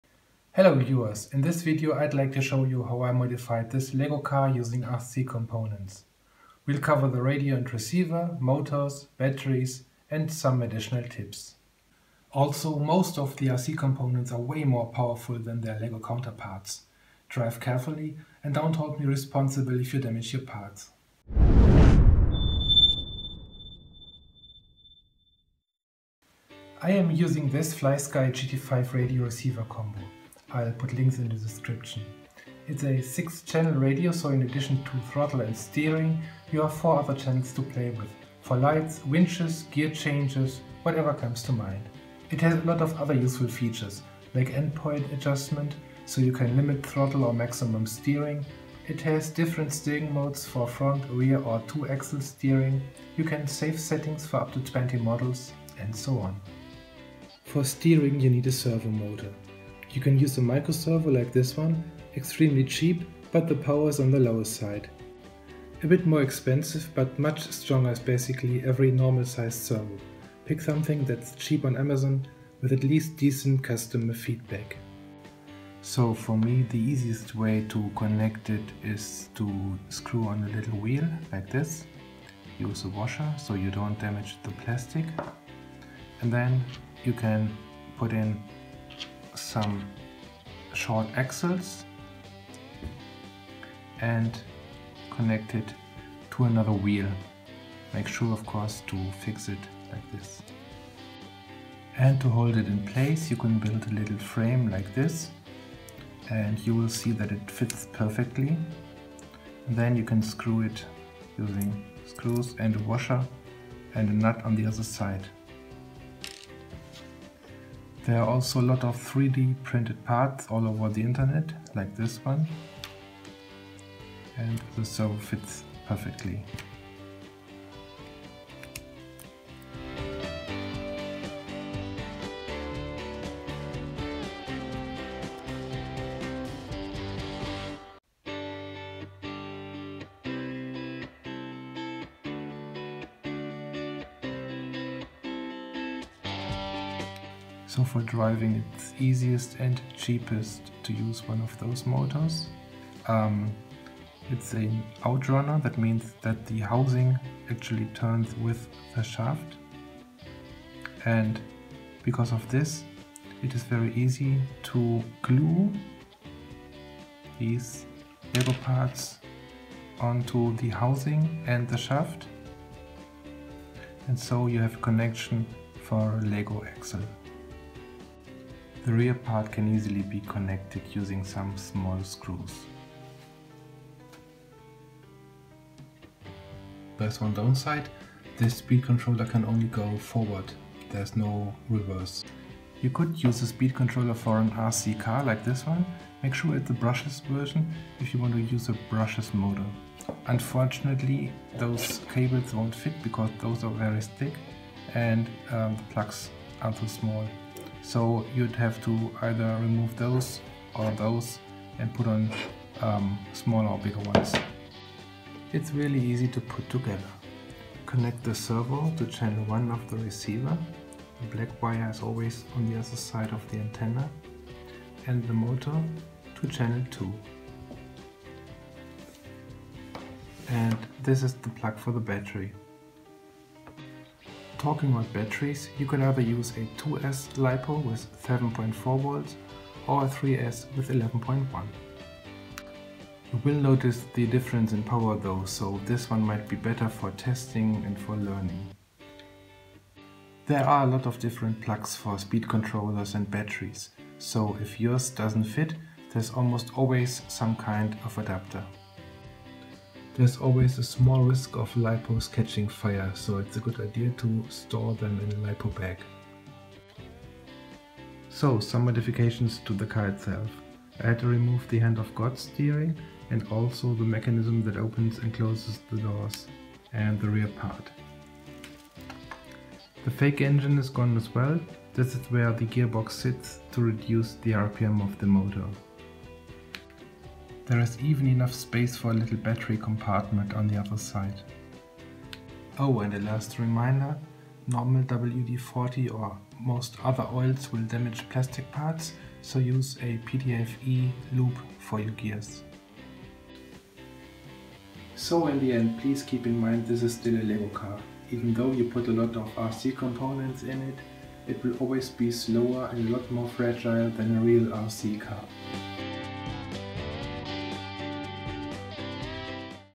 Lego 42039 - Howto mod Lego Technic with RC components
In this short video I'd like to show you how to modify Lego Technic with RC components that are not designed for LEGO.

Please note: The electronics used in this video are not toys. They are rated 14+ by their manufacturers. LiPo-batteries can cause serious damage to people or your house if not handled with care.

Sorry about the terrible light and sound quality.

This is not for LEGO purists! ;-)

Links
LEGO replacement parts and old sets:

RC components:
Main motor
Micro servos